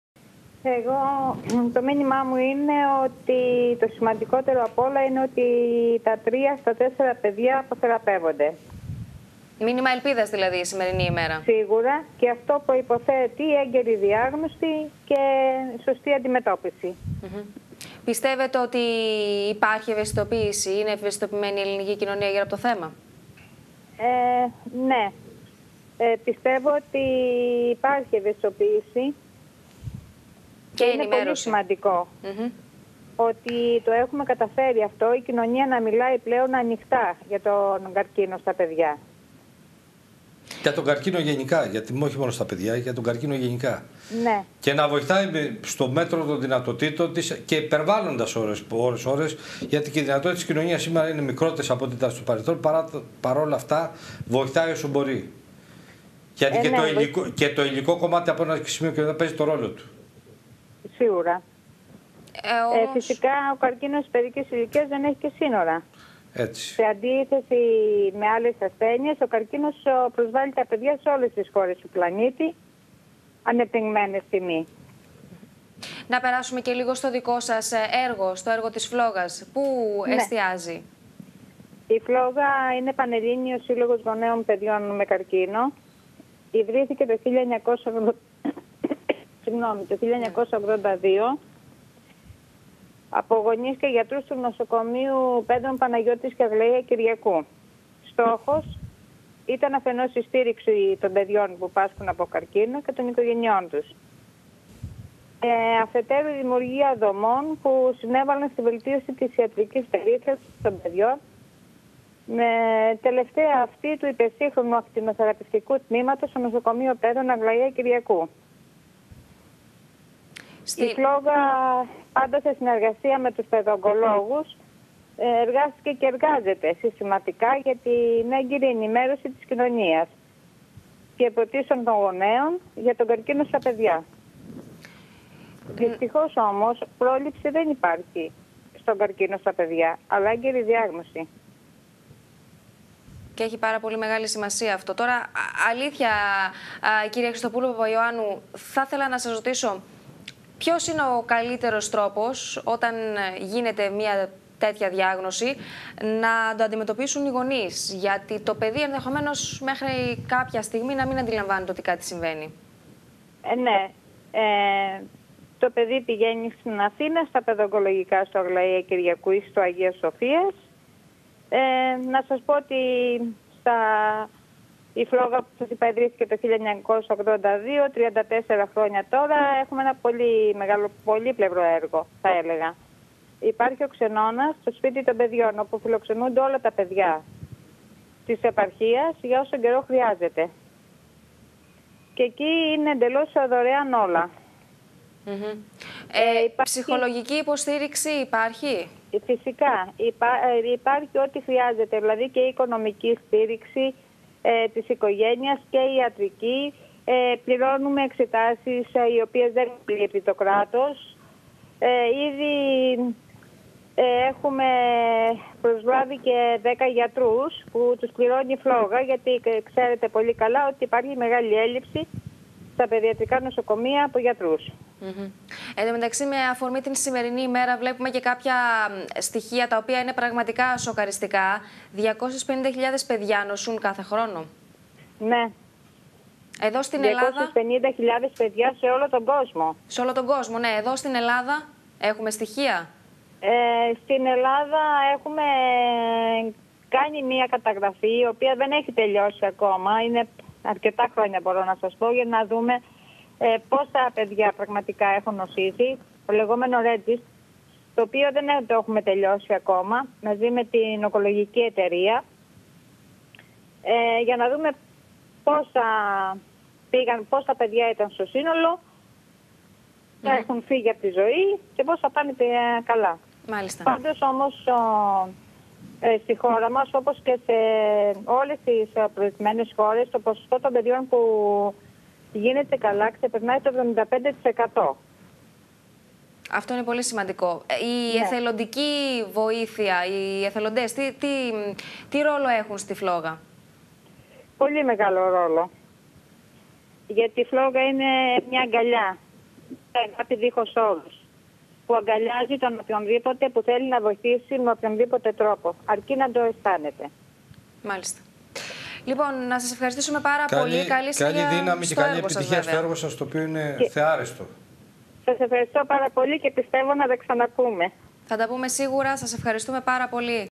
Ο Σύλλογος «ΦΛΟΓΑ» δίπλα στις οικογένειες – Μοιράζονται φόβους και αγωνίες
1) 15 Φεβρουαρίου: Παγκόσμια Ημέρα κατά του Παιδικού Καρκίνου
Παγκόσμια Ημέρα κατά του Παιδικού Καρκίνου: 250.000 παιδιά νοσούν κάθε χρόνο
Τασία Χριστοπούλου – Παπαϊωάννου – Πρόεδρος Συλλόγου «ΦΛΟΓΑ»